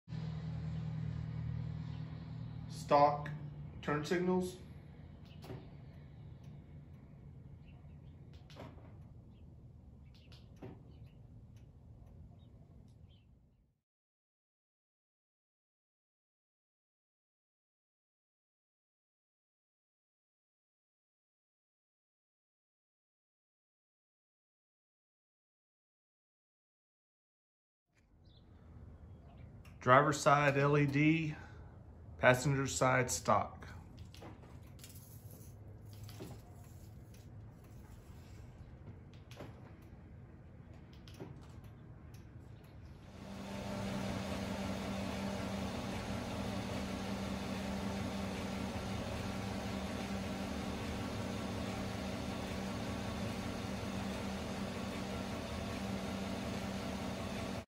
LASFIT Led turn signals 2019 Chevy TrailBoss
Upgrading my stock halogen turn signal bulbs to Lasfit LED turn signals for 2019 Chevy Silverado TrailBoss.